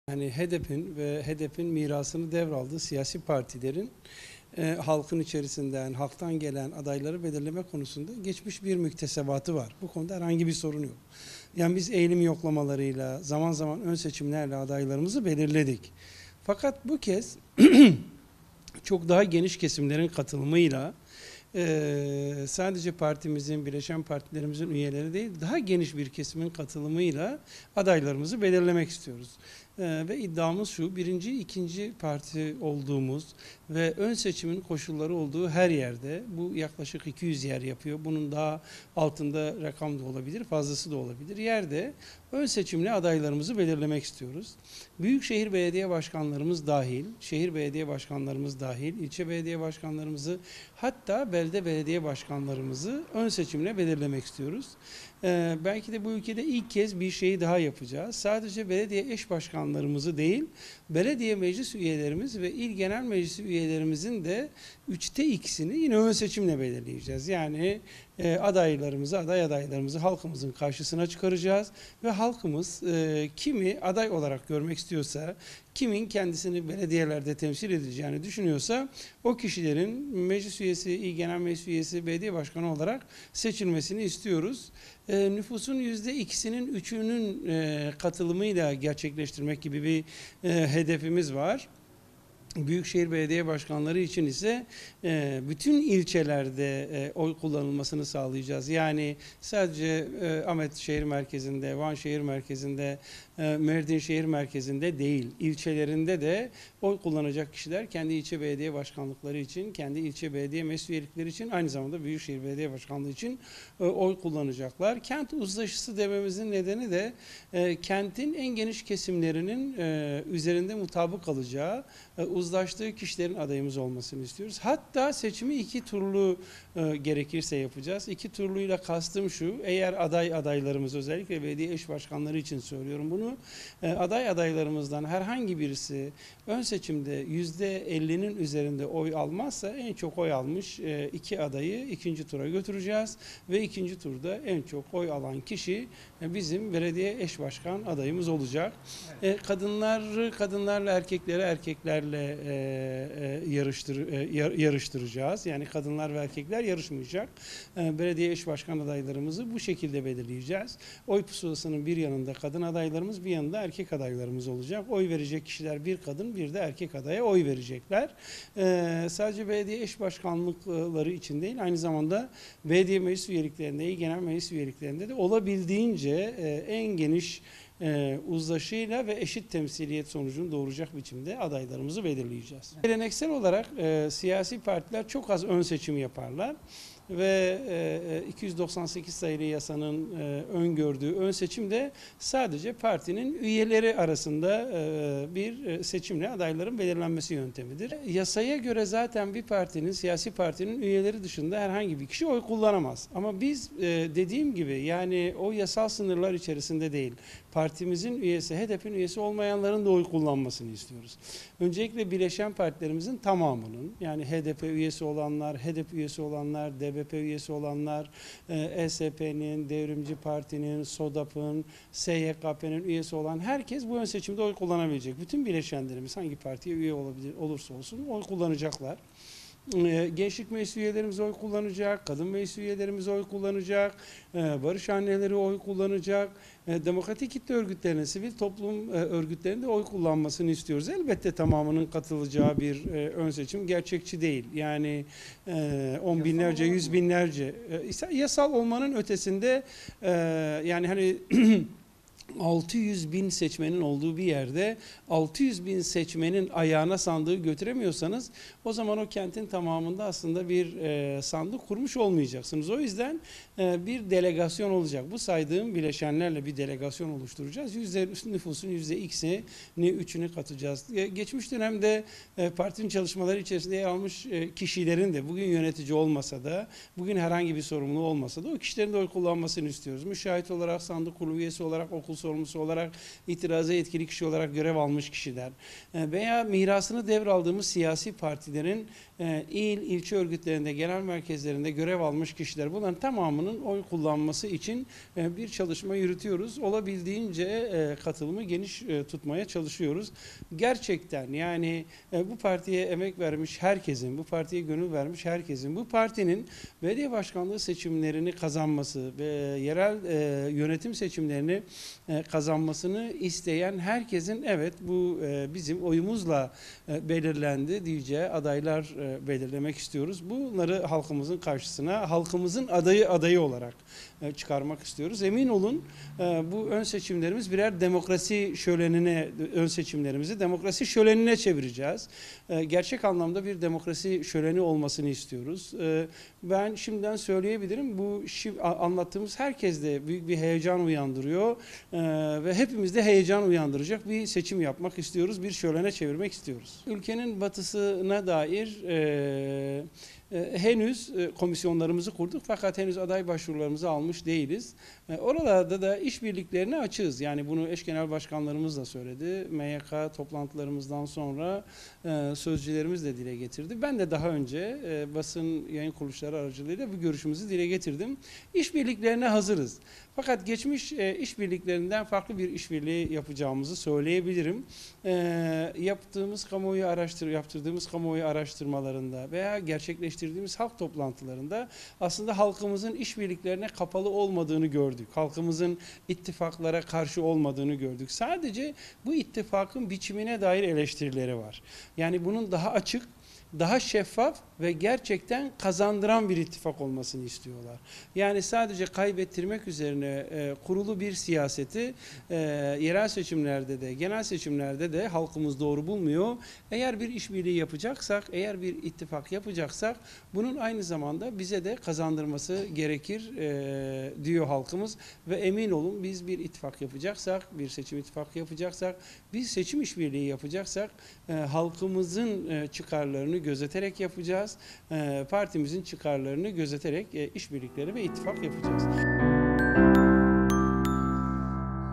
HEDEP YEREL SEÇİMLERDE NASIL BİR STRATEJİ İZLEYECEK?
Kazanabilecekleri kentlerde belediye eşbaşkanları ve meclis adaylarını belirlemek için sandık kuracaklarını aktaran HEDEP Yerel Yönetimlerden Sorumlu Eş Genel Başkan Yardımcısı Rüştü Tiryaki, batı kentlerinde ise “kazandıran bir ittifak” ile halkın karşısına çıkacaklarını söyledi.

Mayıs seçimlerinin ardından Kurdistan ve Türkiye kentlerinde yapılan halk toplantılarında öne çıkan önerilerle yeni dönem politik hattını belirleyen Halkların Eşitlik ve Demokrasi Partisi (HEDEP), bu kapsamda çizdiği yol haritasıyla 31 Mart Yerel Seçimleri’ne giriyor. HEDEP, halkın talepleri doğrultusunda aday belirleme için ön seçim gerçekleştirecek. Böylece, “Kent Uzlaşısı” adı verilen bu ön seçimle hem büyükşehir hem de il ve ilçe belediye eşbaşkanları halkın oylarıyla belirlenecek.
HEDEP Demokratik Yerel Yönetimler Kurulu’ndan Sorumlu Eş Genel Başkan Yardımcısı Mehmet Rüştü Tiryaki, yerel seçimlere ilişkin belirledikleri politik hattı değerlendirdi.